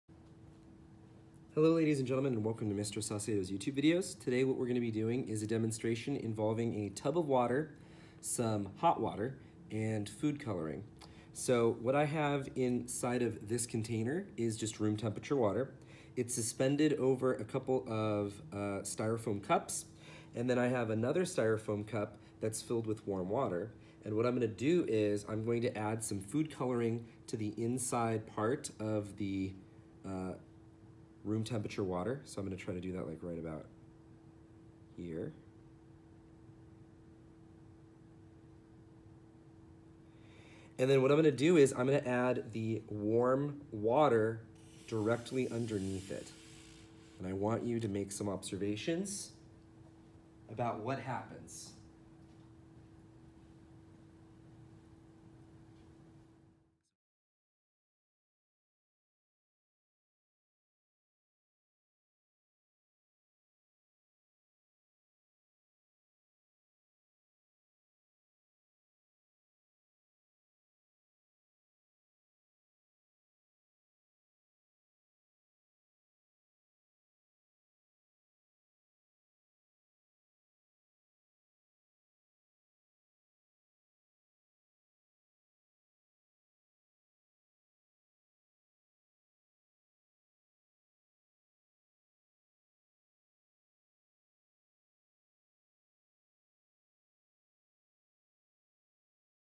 Mystery: Why does the food coloring move?
This video focuses on the mystery of why the food coloring moves. Don't expect the answer to the mystery in this video; this is merely to show what the mystery looks like and to ask follow up questions about it. In order to do this mystery, all you will need is a tub of water, food coloring, a pipette, a cup of hot water, and something to raise the tub of water up above the cup of hot water. I had to speed up the footage in order to make the movement of the food coloring easier to follow.

🧪️ This demonstration is part of a series that is covering our introductory chemistry mysteries. Here is playlist #1
🧪️ This is part of our chemistry in the earth systems curriculum. For the entire playlist of chemistry lessons
Curious about the answer to this? Well, because I use this in class I can't post the answer, but here are some clues!